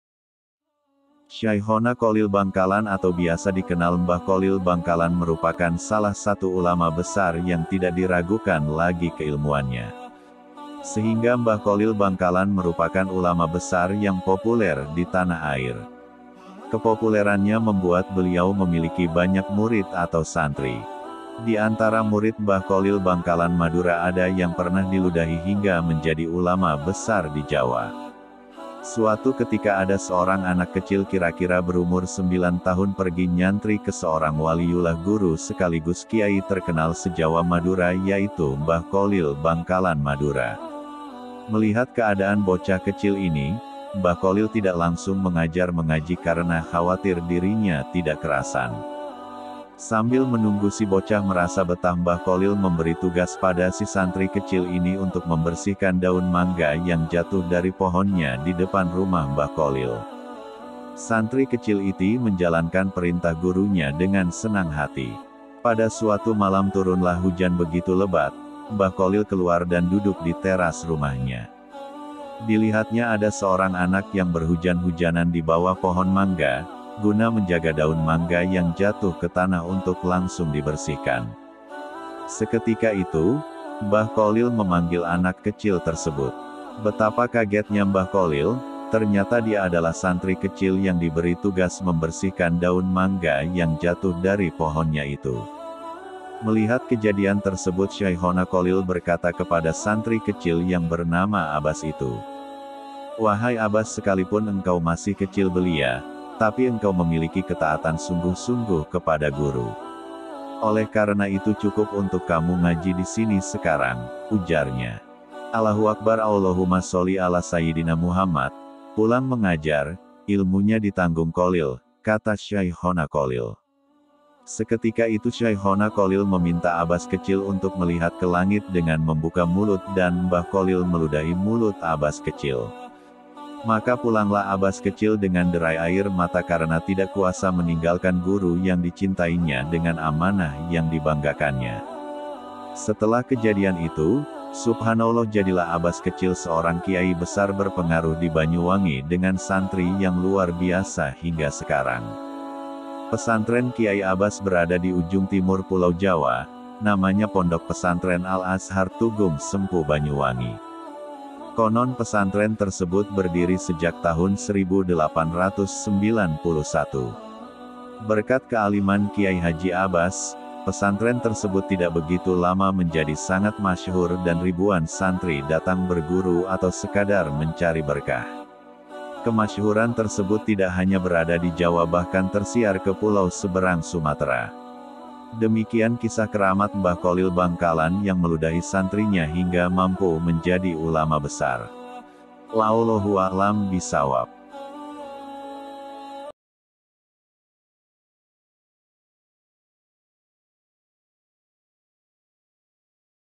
KERAMAT SYAIKHONA KHOLIL|| SANTRI YANG DILUDAHI MENJADI PENDIRI PONDOK BESAR BANYUWANGI#banyuwangi
Kisah santri Syaikhona Kholil Bangkalan yang jadi pendiri pondok pesantren besar di Banyuwangi.